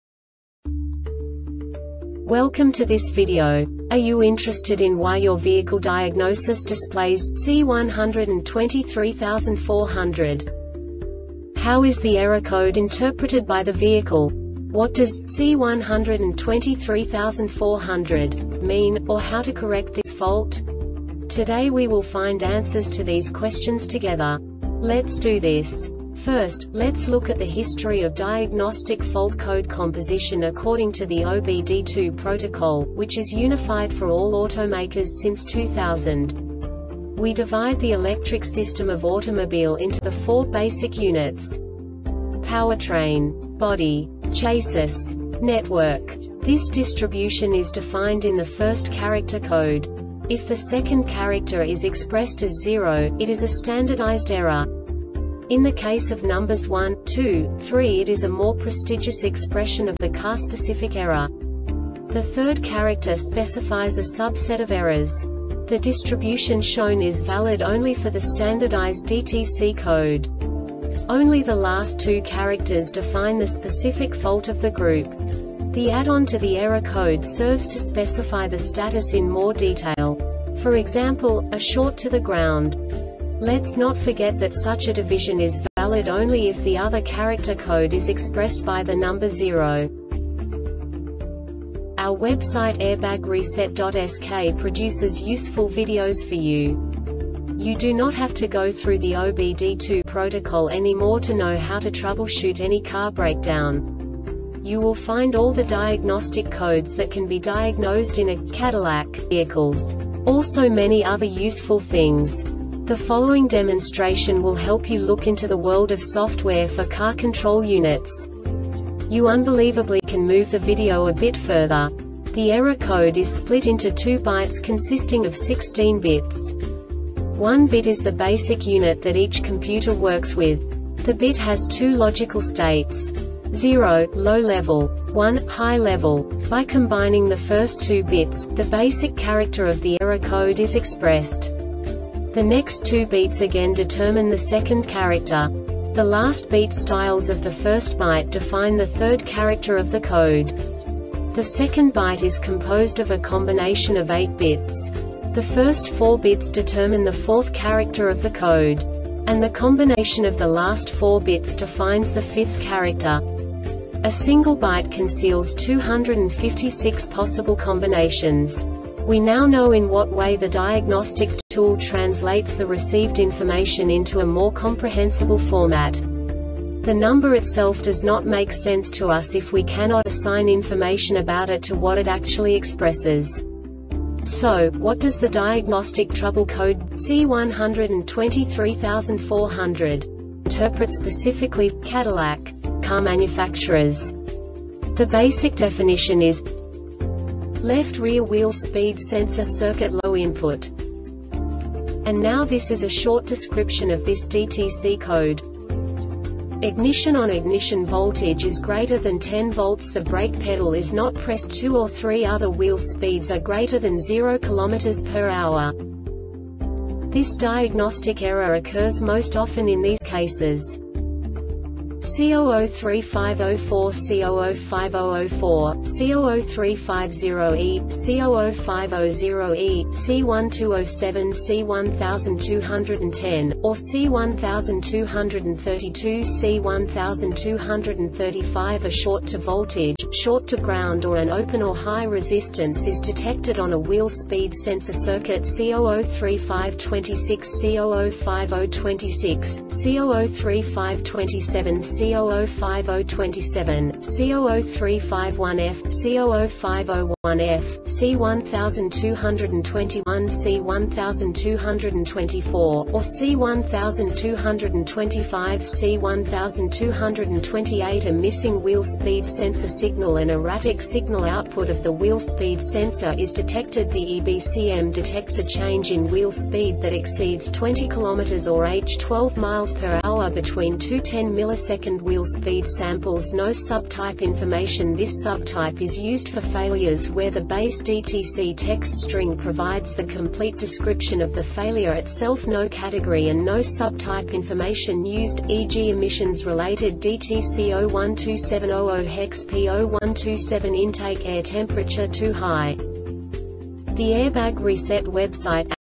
DTC cadillac C1234-00 Short Explanation
Contents:
0:21 Basic DTC analysis according to OBD2 protocol standard.
1:48 Insight into programming
2:58 A diagnostically understandable description of the Diagnostic Trouble Code
3:05 Basic error definition

Make:
Cadillac
Code:
C1234 00
Definition:
Left Rear Wheel Speed Sensor Circuit Low Input
Description:
Cause:
C0035 04 - C0050 04, C0035 0E - C0050 0E, C1207 - C1210, or C1232 - C1235
A short to voltage, short to ground or an open or high resistance is detected on a wheel speed sensor circuit.
C0035 26 - C0050 26, C0035 27 - C0050 27, C0035 1F - C0050 1F, C1221 - C1224, or C1225 - C1228
A missing wheel speed sensor signal.
An erratic signal output of the wheel speed sensor is detected.
The EBCM detects a change in wheel speed that exceeds 20 km or h (12 mph) between two 10 millisecond wheel speed samples.